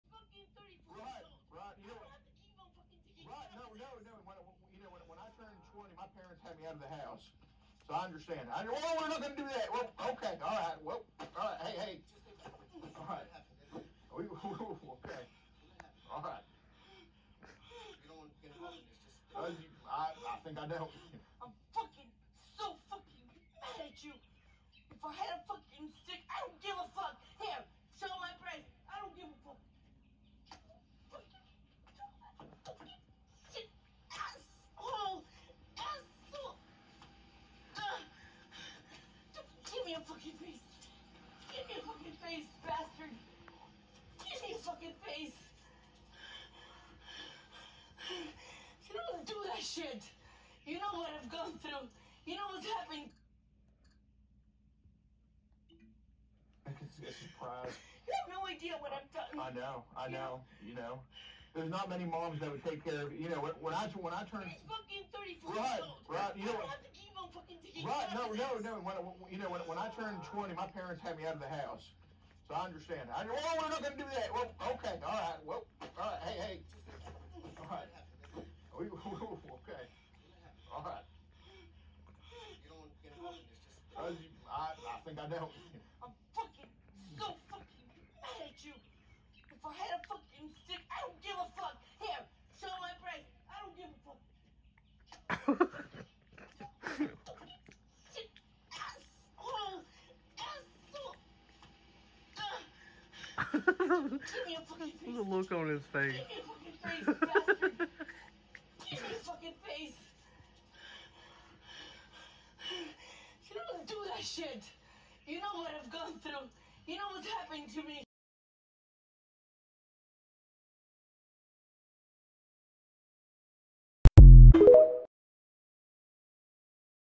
The look on Gordon’s Face when this mother beats her son up is priceless lol
Alex informs This guys mother he has been talking to under age kids online and when she starts beating the crap out of her son and her tits pop out the look on Gordons face is priceless!!! Lol never piss off a latino woman or hispanic woman lol they dont play they will beat yo azz lol 🤣 new news delphiafterdawn fight drama funny predator exposed boobies 🤣